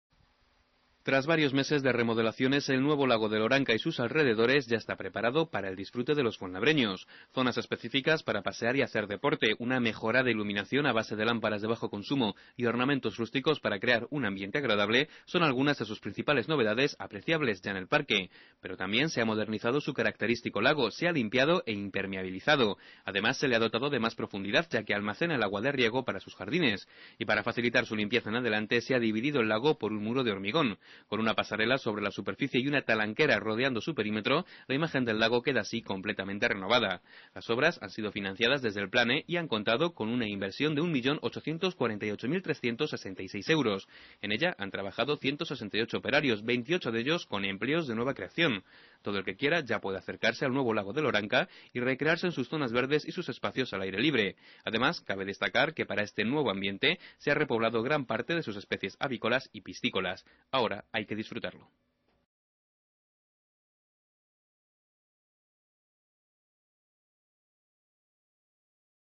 Los fuenlabreños ya pueden disfrutar del remodelado lago de Loranca
Remodelada gracias a la inversión procedente del Plan E, el parque del Lago de Loranca ha impermeabilizado su estanque y le ha datado de mayor profundidad. Además cuenta ahora con una mejorada iluminación, se han creado espacios específicos para pasear y se han repoblado sus especies avícolas. Obras para las que se ha destinado un importe de más de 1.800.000 euros y que han creado cerca de 170 puestos de trabajo.